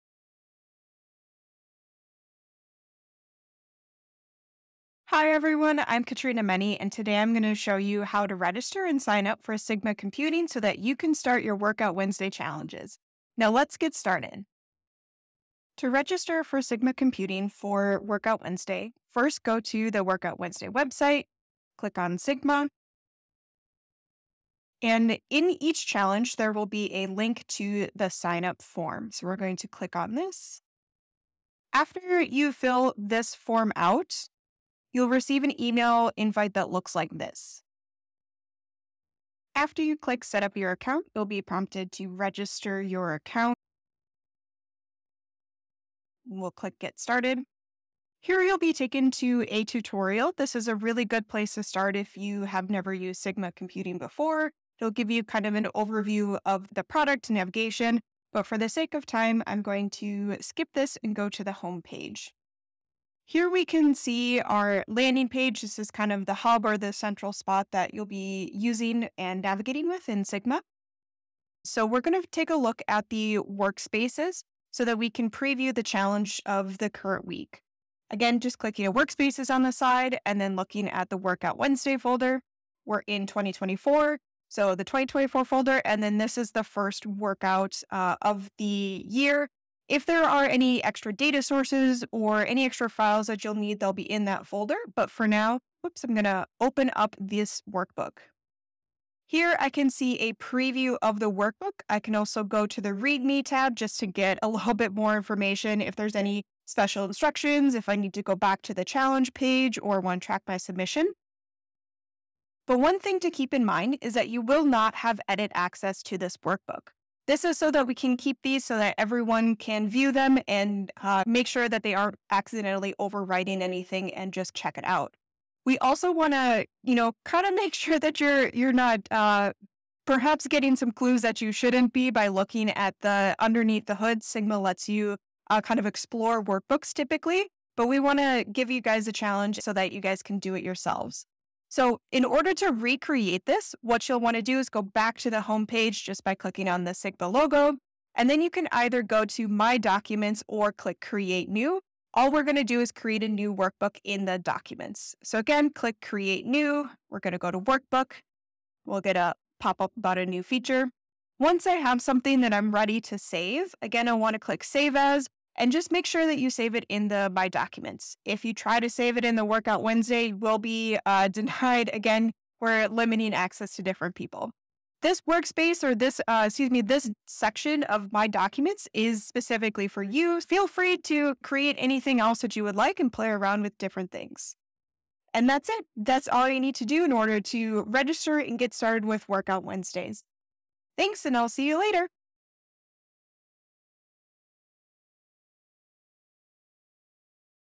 How to Register & Get Started with Sigma Workout Wednesday
A walkthrough of registering and starting with Sigma Computing Workout Wednesday (WOW) challenges.

WOW are bite-sized weekly challenges designed to help learn data analytics platforms like Sigma Computing.